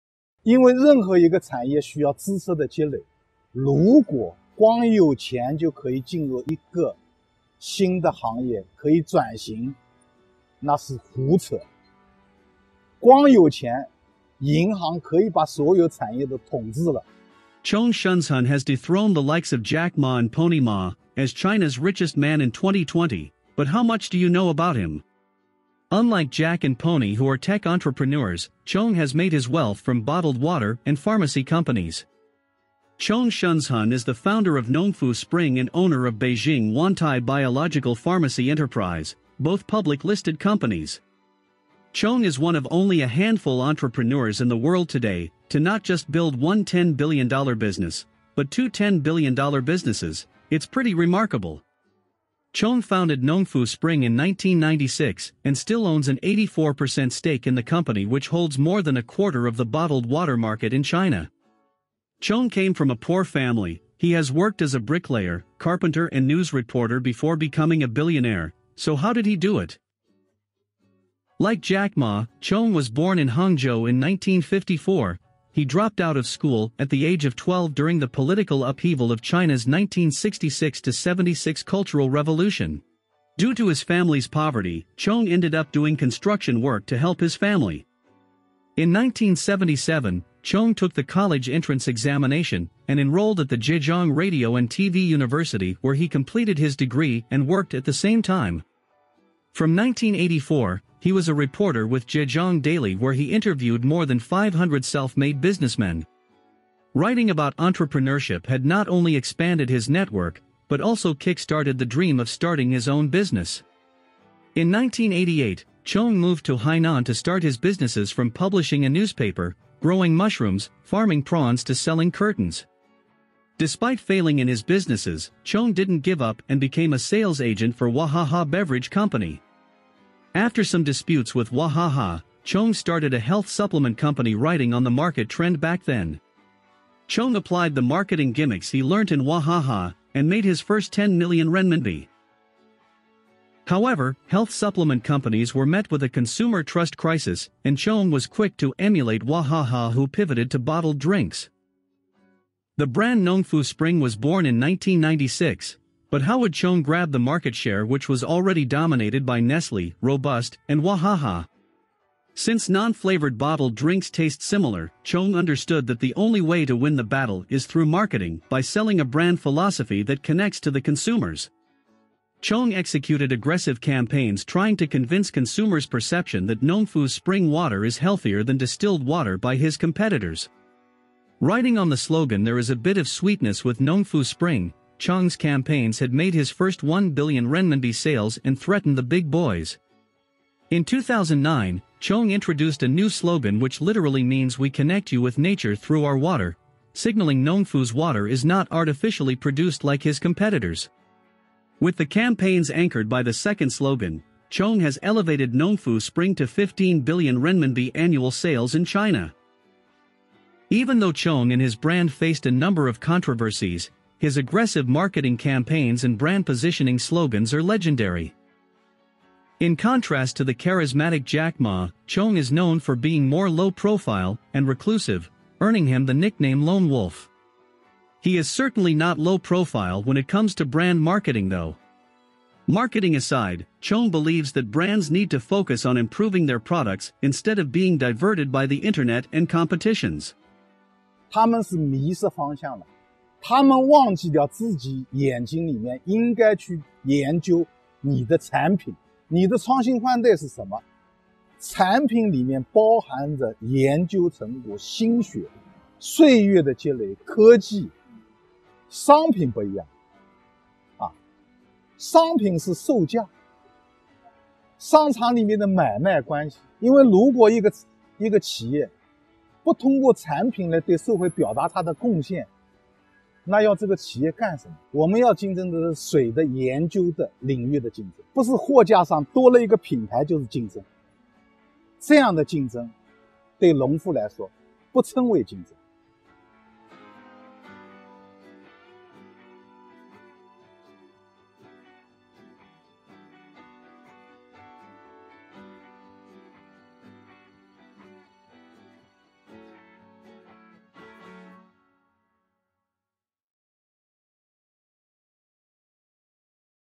Zhong Shanshan (钟睒睒), Founder of Nongfu Spring - The Lone Wolf
You might not have heard of his name, but Zhong Shanshan (钟睒睒) has become the richest man in China and even Asia in 2020/21, dethroning the likes of Jack Ma (Alibaba) and Pony Ma (Tencent). Unlike Jack Ma, Zhong is very low profile, earning him the nickname “lone wolf”.

Find out how Zhong came from a poor family in Hangzhou, became a news reporter, failed in a few businesses, and rise to the top with not just one, but two $10 billion businesses.

Chapters:
0:00 Intro to Zhong Shanshan
0:17 Zhong is the richest man in China
1:11 The background of Zhong Shanshan
1:39 Inspired while working as a reporter
2:10 Zhong started a health supplement company
2:23 Pivoted to bottled drinks, Nongfu Spring was born
2:42 Marketing is the way to win in the competitive market
3:52 Zhong is a strong believer in product development
4:01 Zhong's take on competitions & e-commerce

Footage credit:
• 凤凰网
• Youku.com
• CCTV.com
• Pexels.com
• Videezy.com
• Videvo.net